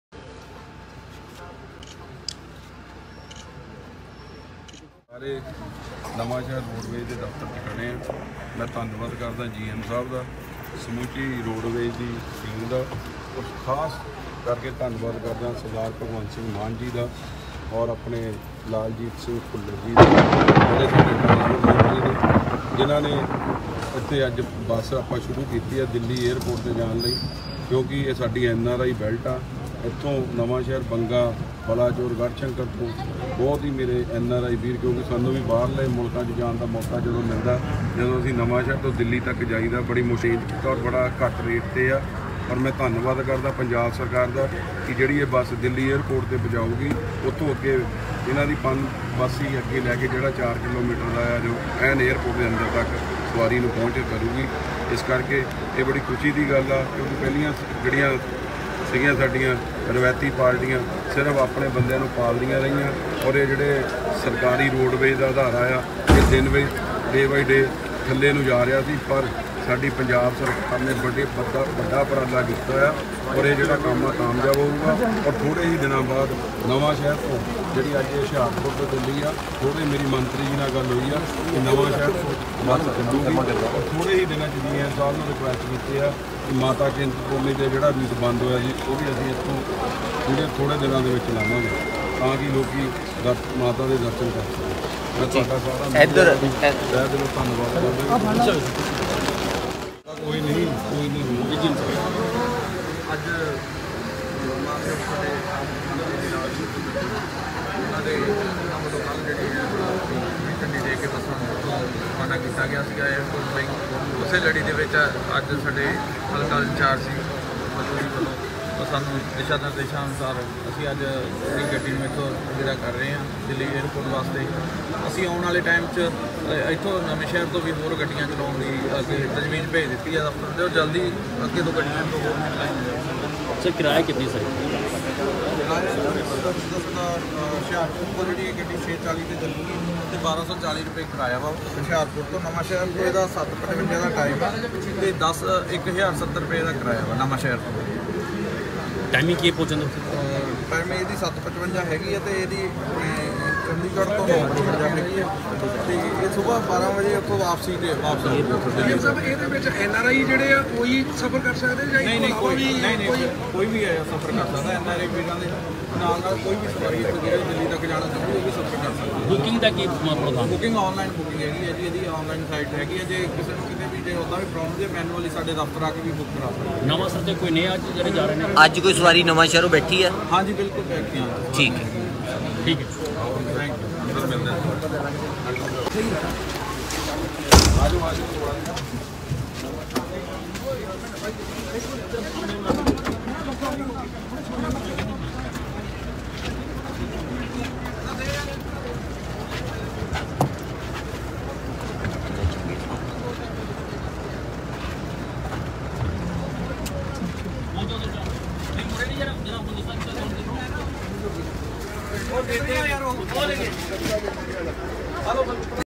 ਦਿੱਲੀ Airport ਜਾਣ ਵਾਲੀ AC ਬਸ ਪਹੁੰਚੀ ਨਵਾਂਸ਼ਹਿਰ ! ਜਲਦ ਹੀ ਮਾਤਾ ਚਿੰਤਪੁਰਨੀ ਨੂੰ ਸਿੱਧੀ ਬਸ ਕਰਾਵਾਂਗੇ ਸ਼ੁਰੂ:ਬੱਲੂ
ਦਿੱਲੀ Airport ਜਾਣ ਵਾਲੀ AC ਬਸ ਪਹੁੰਚੀ ਨਵਾਂਸ਼ਹਿਰ , ਮੌਕੇ ਤੇ ਪਹੁੰਚੇ ਆਪ ਆਗੂ ਲਲਿਤ ਮੋਹਨ ਪਾਠਕ ਨੇ ਕਿਹਾ ਜਲਦ ਹੀ ਮਾਤਾ ਚਿੰਤਪੁਰਨੀ ਨੂੰ ਵੀ ਸਿੱਧੀ ਬਸ ਕਰਾਵਾਂਗੇ ਸ਼ੁਰੂ।